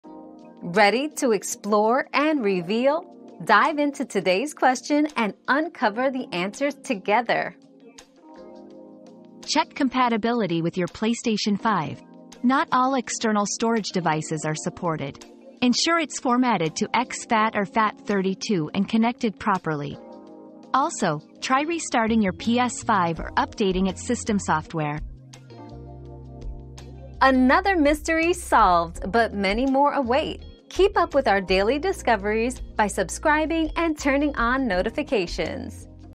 Why isn't my PlayStation 5 recognizing external storage devices?
PlayStation 5 External Storage Issues: Troubleshooting Guide 👉 PS5 Storage Guide 👉 Learn how to troubleshoot PlayStation 5 external storage recognition issues. Check compatibility, format correctly, and update system software for a smooth experience.

Laura S. Harris (2024, June 1.) Why isn't my PlayStation 5 recognizing external storage devices?
    🌐 AskAbout.video